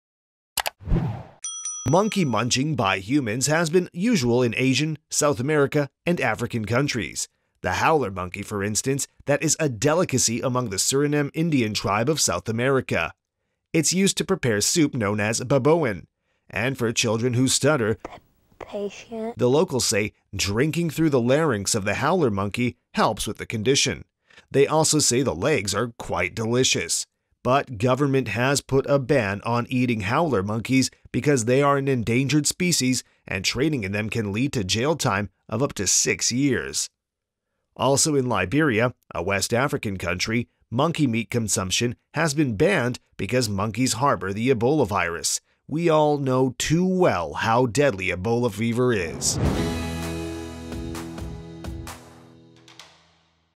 Eating Monkey meat | howler monkey in Suriname
Forget about the adorable baby monkey or even a pet monkey, these guys enjoy eating monkey meat.
The howler monkey specifically is considered a delicacy among the Suriname tribe dating back to the earliest native tribes.
Hunting these monkeys is illegal, yet bush meat is a great source of protein for the locals.

They are an endangered species here and in Liberia, monkey munching was banned because of possible Ebola transmission

More interesting things you don't know